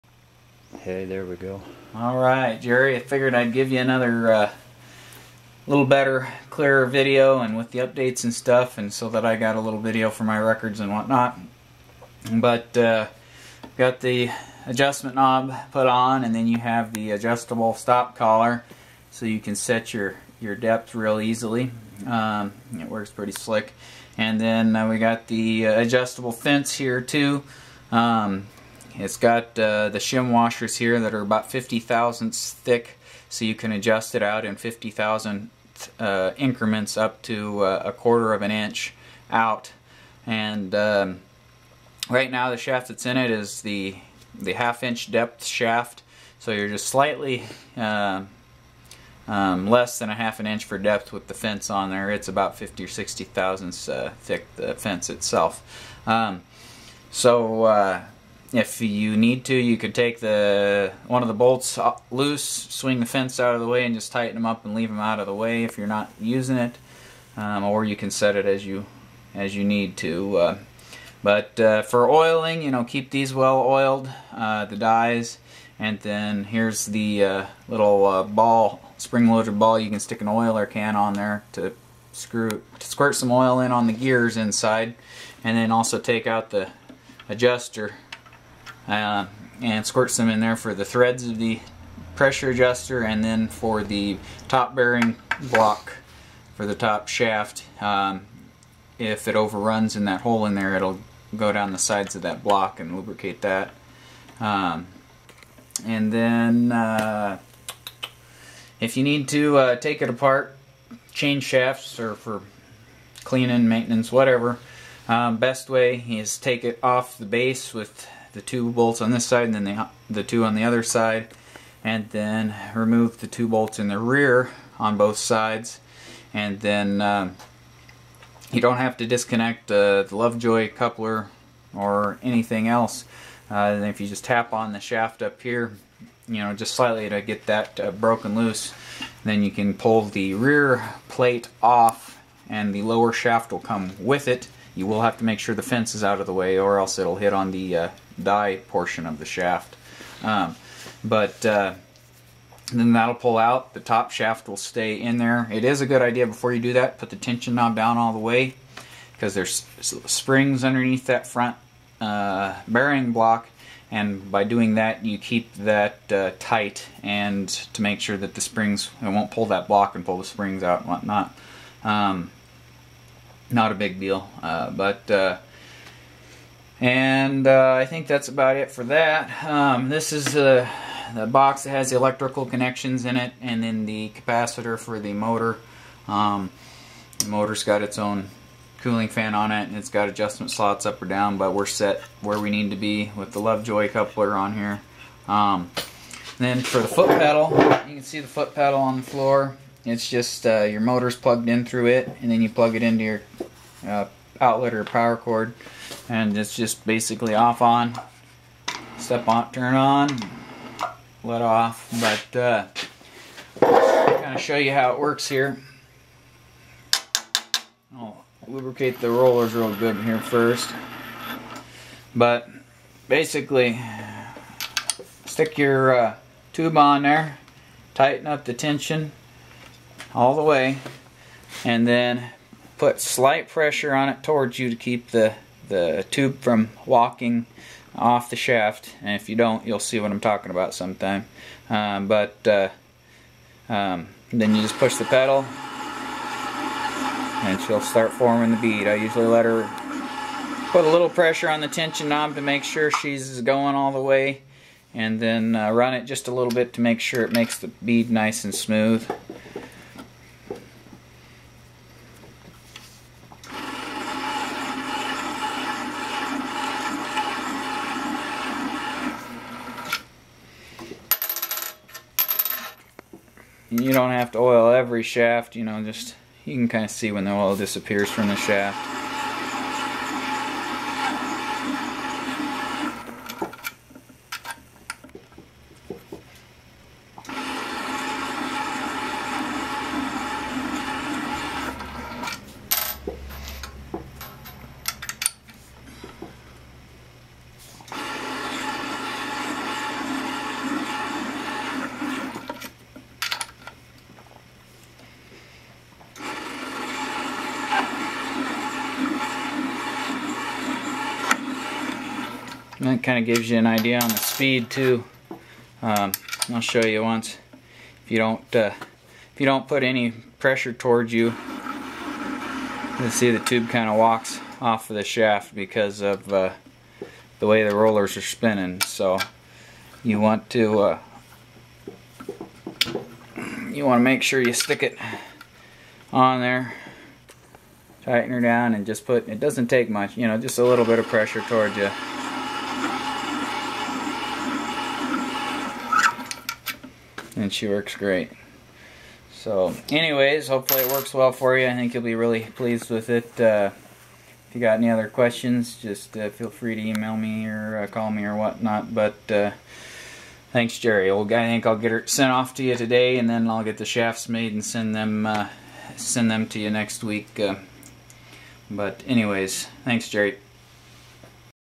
Custom Tubing Beader Tool Jerry #2
Overview and Demo Video of the 2nd Tubing Tool Custom made for one of our customers Jerry.  This was a video made for him to show operation and maintenance, but figured i would make it public now for others to see if interested.

He needed a tube beading tool for production of an item that needed specific dimensions that were not available on standard tubing beaders already available and it needed to work quickly and easily on the stainless steel tubing.  He discussed his needs for the tool and I designed and built one to suit his needs.